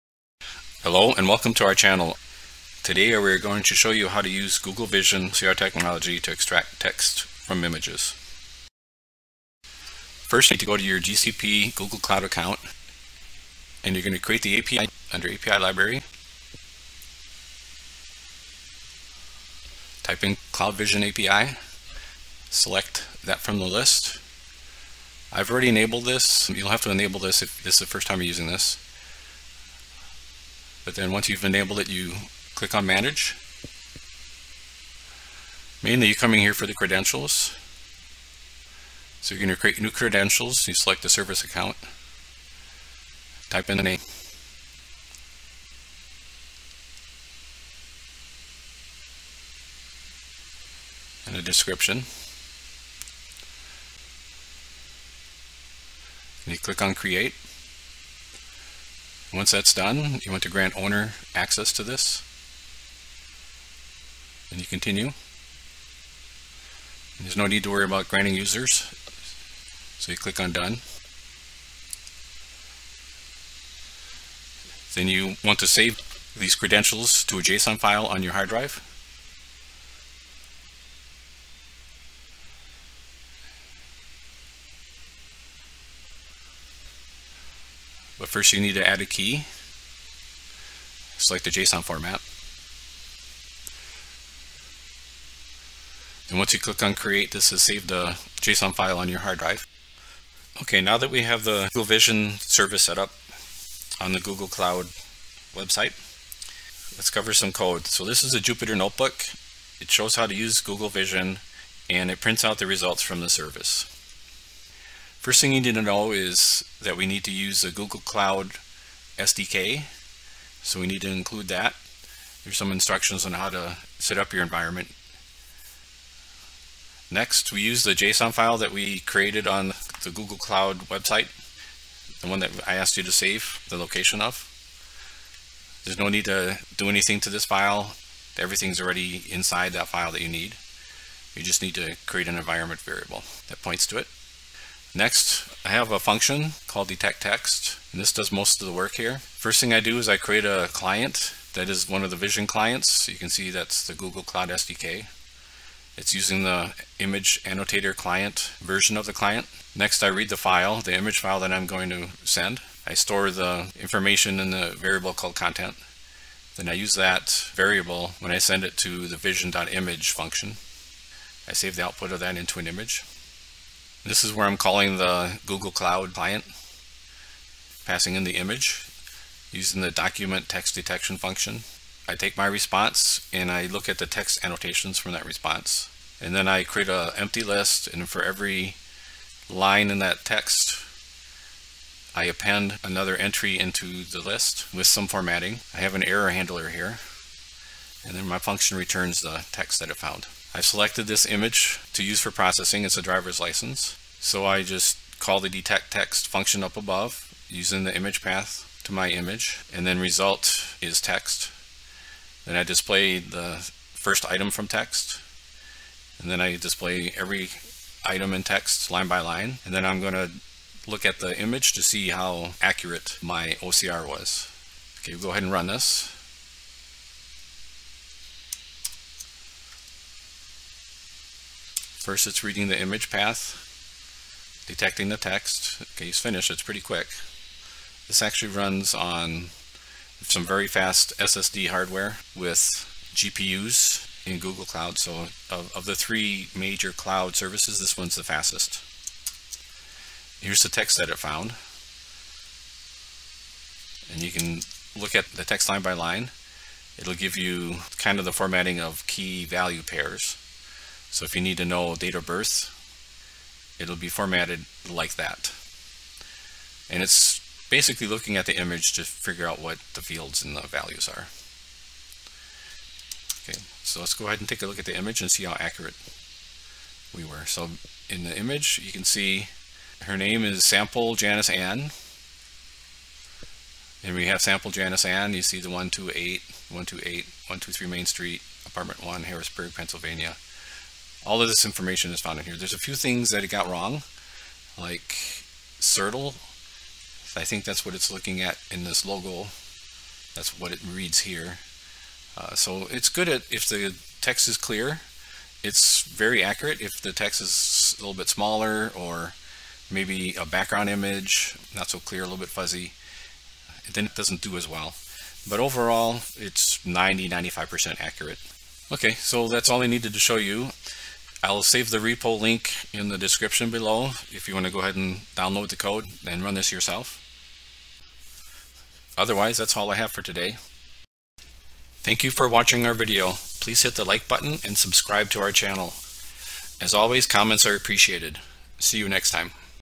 Google Cloud Vision API for OCR Text Extraction: Tutorial Google Cloud Vision AI
This tutorial will demonstrate how to extract text from an image with high accuracy using the Google Vision AI API with Python.

Google Vision API for OCR Text Extraction: Tutorial Google Vision AI